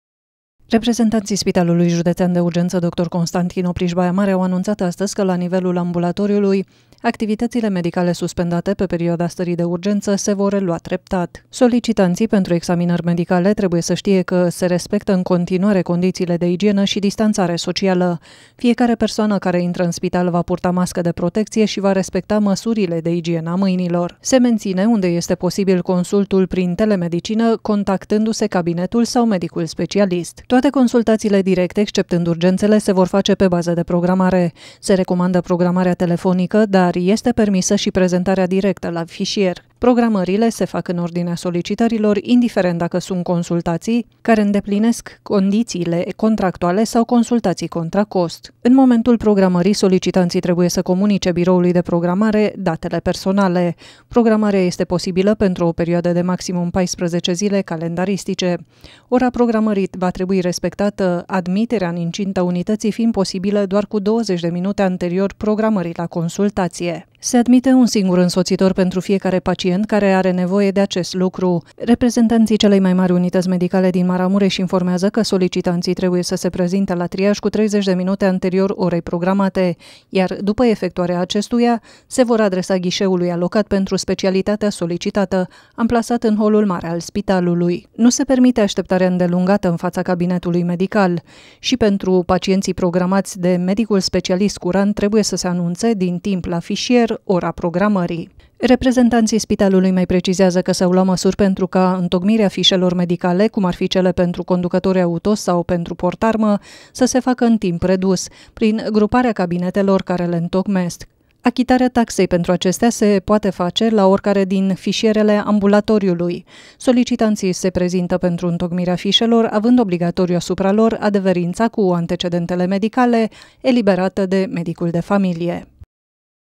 Se reia activitatea medicală în ambulatoriu
Activitățile medicale ale ambulatoriului Spitalului Județean din Baia Mare se vor relua încet o dată cu suspendarea stării de urgență.
Reprezentanții celei mai mari unități medicale din Maramureș precizează insă că se vor face pe bază de programare consultațiile directe.